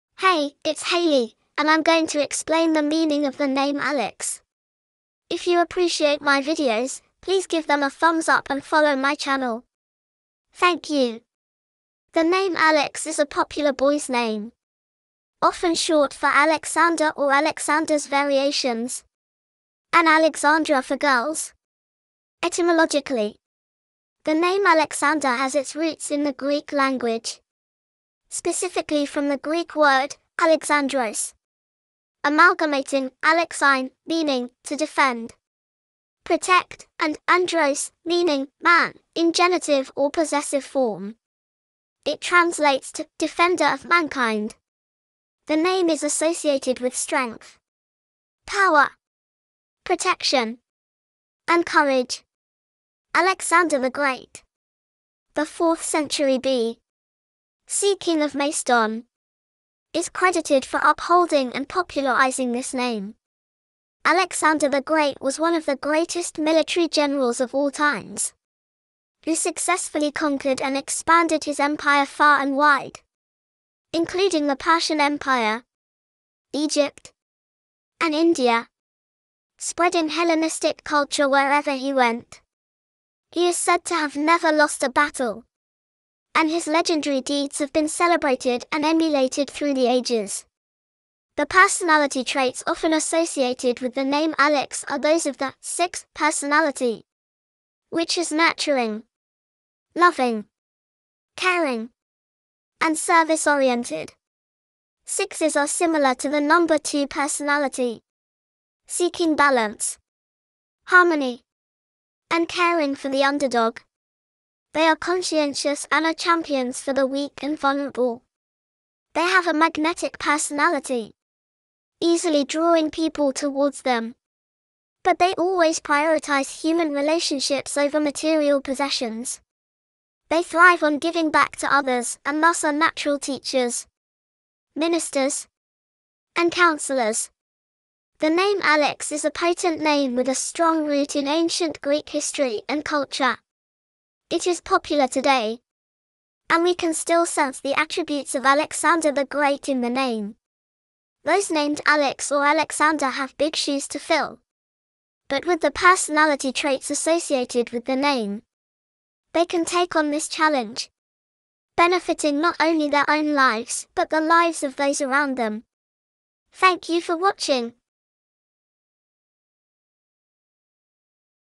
Meaning of boy name: Alex - Name History, Origin and Popularity
Hey, it's Haley, and I'm going to explain the meaning of the name Alex.